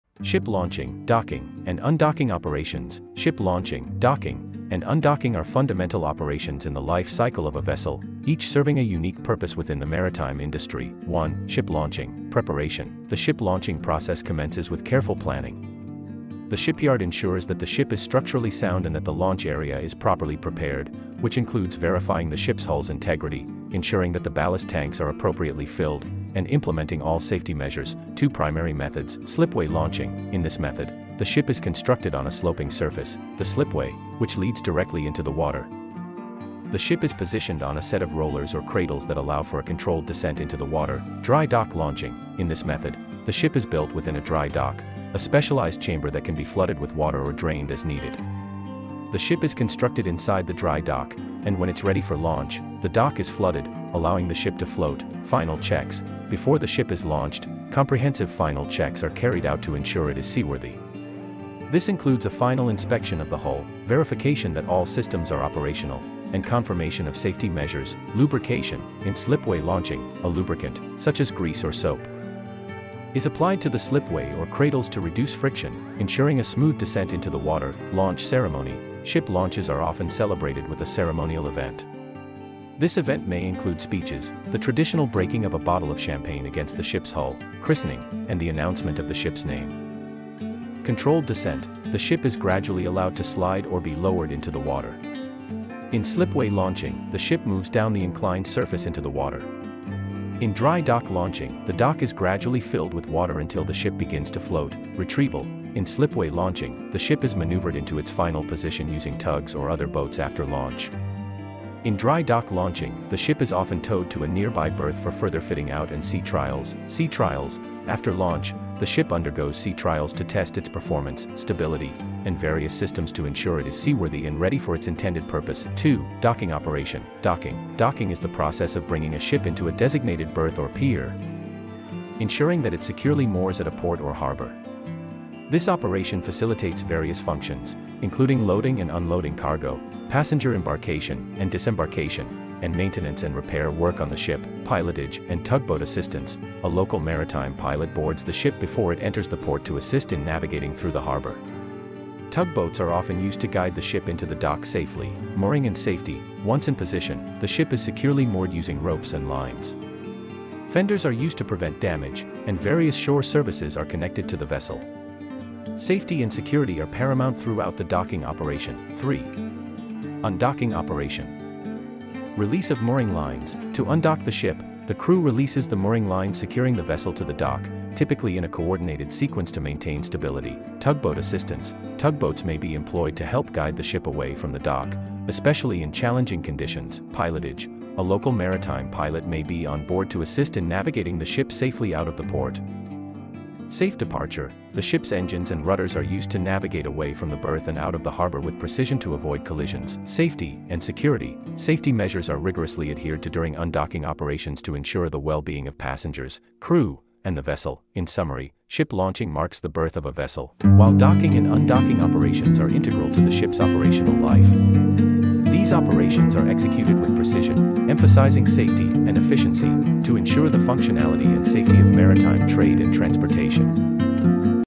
Ship Launching, Docking, and Undocking: Where Engineering Meets the Sea
Ship Launching, Docking, and Undocking Operations:
Ship launching, docking, and undocking are fundamental operations in the life cycle of a vessel, each serving a unique purpose within the maritime industry.
1. Ship Launching:
Preparation: The ship launching process commences with careful planning. The shipyard ensures that the ship is structurally sound and that the launch area is properly prepared, which includes verifying the ship's hull's integrity, ensuring that the ballast tanks are appropriately filled, and implementing all safety measures.
Two Primary Methods:
Slipway Launching: In this method, the ship is constructed on a sloping surface, the slipway, which leads directly into the water. The ship is positioned on a set of rollers or cradles that allow for a controlled descent into the water.
Dry Dock Launching: In this method, the ship is built within a dry dock, a specialized chamber that can be flooded with water or drained as needed. The ship is constructed inside the dry dock, and when it's ready for launch, the dock is flooded, allowing the ship to float.
Final Checks: Before the ship is launched, comprehensive final checks are carried out to ensure it is seaworthy. This includes a final inspection of the hull, verification that all systems are operational, and confirmation of safety measures.
Lubrication: In slipway launching, a lubricant, such as grease or soap, is applied to the slipway or cradles to reduce friction, ensuring a smooth descent into the water.
Launch Ceremony: Ship launches are often celebrated with a ceremonial event. This event may include speeches, the traditional breaking of a bottle of champagne against the ship's hull (christening), and the announcement of the ship's name.
Controlled Descent: The ship is gradually allowed to slide or be lowered into the water. In slipway launching, the ship moves down the inclined surface into the water. In dry dock launching, the dock is gradually filled with water until the ship begins to float.
Retrieval: In slipway launching, the ship is maneuvered into its final position using tugs or other boats after launch. In dry dock launching, the ship is often towed to a nearby berth for further fitting out and sea trials.
Sea Trials: After launch, the ship undergoes sea trials to test its performance, stability, and various systems to ensure it is seaworthy and ready for its intended purpose.
2. Docking Operation:
Docking: Docking is the process of bringing a ship into a designated berth or pier, ensuring that it securely moors at a port or harbor. This operation facilitates various functions, including loading and unloading cargo, passenger embarkation and disembarkation, and maintenance and repair work on the ship.
Pilotage and Tugboat Assistance: A local maritime pilot boards the ship before it enters the port to assist in navigating through the harbor. Tugboats are often used to guide the ship into the dock safely.
Mooring and Safety: Once in position, the ship is securely moored using ropes and lines. Fenders are used to prevent damage, and various shore services are connected to the vessel. Safety and security are paramount throughout the docking operation.
3. Undocking Operation:
Release of Mooring Lines: To undock the ship, the crew releases the mooring lines securing the vessel to the dock, typically in a coordinated sequence to maintain stability.
Tugboat Assistance: Tugboats may be employed to help guide the ship away from the dock, especially in challenging conditions.
Pilotage: A local maritime pilot may be on board to assist in navigating the ship safely out of the port.
Safe Departure: The ship's engines and rudders are used to navigate away from the berth and out of the harbor with precision to avoid collisions.
Safety and Security: Safety measures are rigorously adhered to during undocking operations to ensure the well-being of passengers, crew, and the vessel.
In summary, ship launching marks the birth of a vessel, while docking and undocking operations are integral to the ship's operational life. These operations are executed with precision, emphasizing safety and efficiency, to ensure the functionality and safety of maritime trade and transportation.